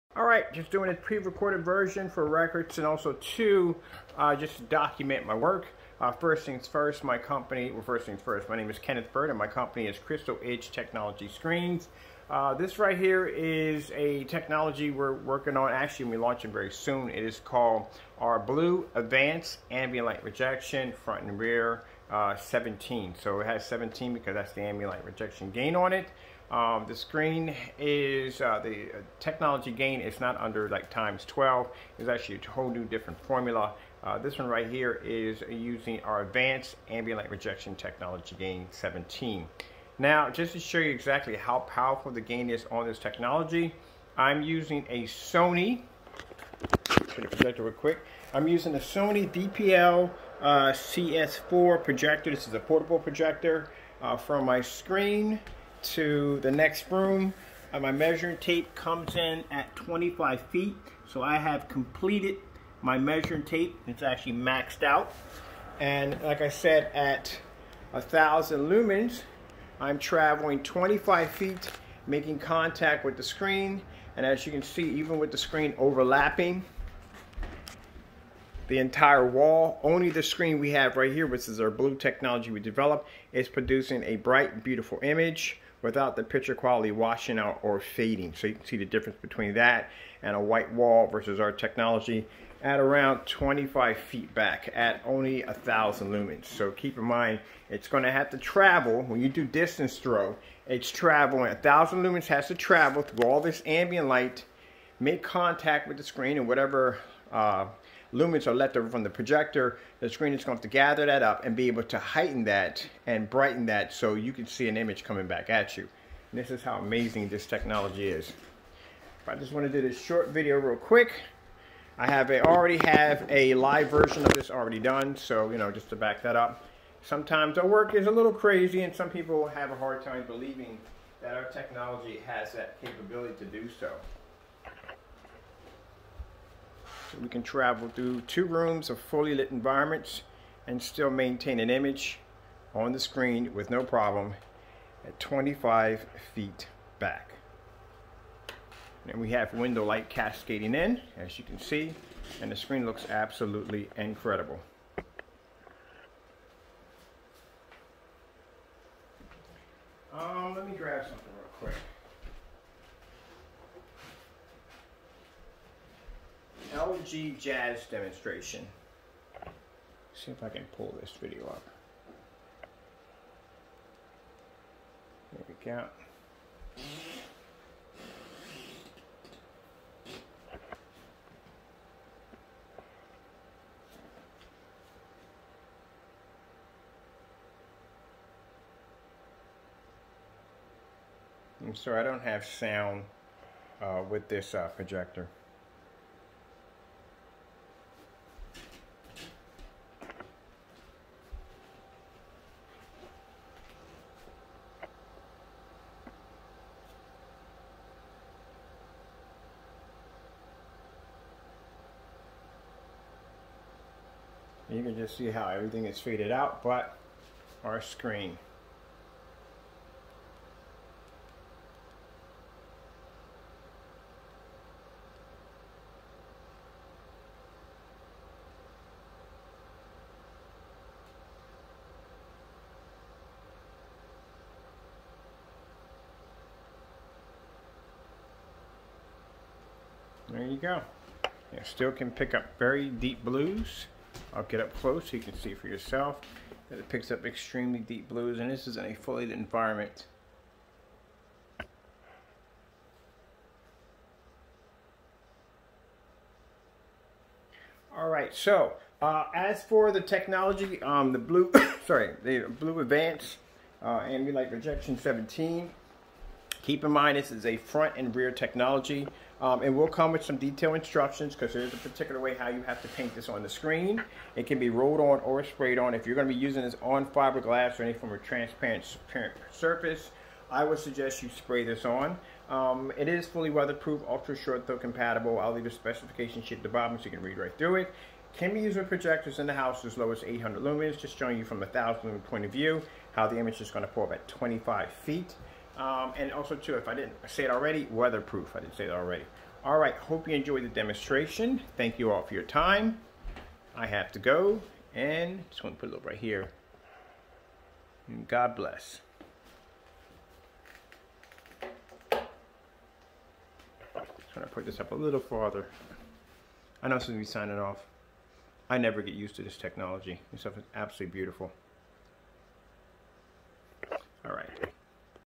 OUR Incredible new 2021 ADV ALR technology gain 17 on 1000 lumens 25ft back lit rooms!
Attention due to covid-19 we can not ship overseas!

New Blue ADV - ALR  front & rear 17 screen paint.
Lumens indoor 800 Lumens outdoor 1000 Note best used in the hours of 6pm -7pm do not use in direct sunlight.
NEW  ADV - ALR technology gain X 17 100% Color Coding Technology.
Color enhancement technology X 17
ULTRA 3D and ULTRA HDR Ready.
Compatible with DLP, LED, LCD, LASER, UST.

Insane black levels coding. Amazing white levels and breathtaking brilliant vivid colors coding technology. Resolution compatibility: 4K, 1080p, 720p,

Fully weatherproof for outdoor home cinema.

Apply on application: Roll on or spray on NOTE do not use foam rollers. Drying time with fan 30 minutes. Low odor, low VOC, Eco friendly, NO sanding or priming needed only if using fabric. KEEP OUT OF REACH FROM CHILDREN AND PET!

Surfaces: wood, vinyl, PVC, glass note this is a front projection screen paint, drywall, brick, cement, plexiglass and other plastic, All kinds of fabric PLEASE NOTE! when painting fabric priming the surface two times will be needed. Sheet metal, Do not paint on rough surfaces. Only 1 coat needed 2 if needed streaks proof, EZ to paint.

PRICE AND SCREEN SIZES:
Screen size: 1 QT screen size 100" - 126" Ratio 16:9, 16:10 , 2.35:1 Price $189
Screen size: 2 QT screen size 100" - 150" Ratio 16:9, 16:10 , 2.35:1  Price $227
Screen size: 1 GAL screen size 100" - 198" Ratio 16:9, 16:10 , 2.35:1 Price $289

Clean up: Hot water or warm mild soaps only needed.

New color coding technology with color enhancement technology bring new life to old out dated projectors without the need to break the bank on a new projector. Ez to paint only 1 - 2 coat needed 2 coats if needed. \

New  ADV - ALR technology gain X 17  Take in more ambient light inside or out without the worries of a washout or dull picture.

Projector used
SONY VPL CS4  Portable projector.
SVGA, 720P, 600X800
Lumens 1000